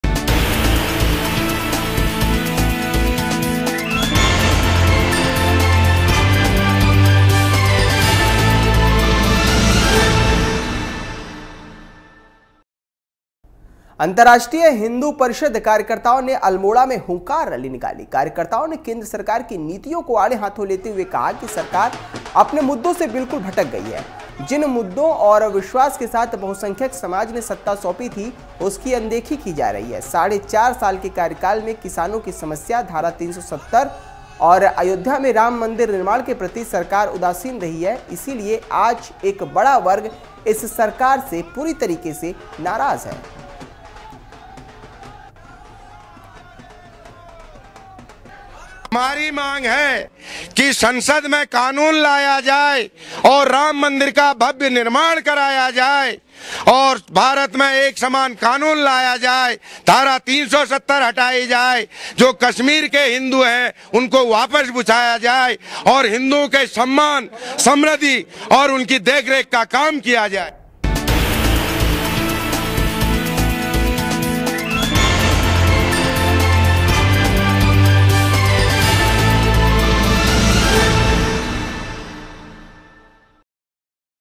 Almora : IHP कार्यकर्ताओं ने निकाली हुंकार रैली ।
अंतर्राष्ट्रीय हिंदू परिषद कार्यकर्ताओं ने अल्मोड़ा में हुंकार रैली निकाली. कार्यकर्ताओं ने केन्द्र सरकार की नीतियों को आड़े हाथों लेते हुए कहा कि सरकार अपने मुद्दे से भटक गई है. जिन मुद्दों और विश्वास के साथ बहुसंख्यक समाज ने सत्ता सौंपी थी उसकी अनदेखी की जा रही है.